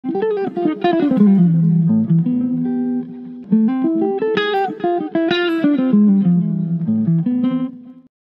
Jazz Fusion Lick With TAB - Rodrigo Gouveia
Jazz Fusion Lick by Rodrigo Gouveia.
e aprenda guitarra Neo Soul e Jazz Fusion. São mais de 90 vídeos, suporte para duvidas, material de apoio e muito mais.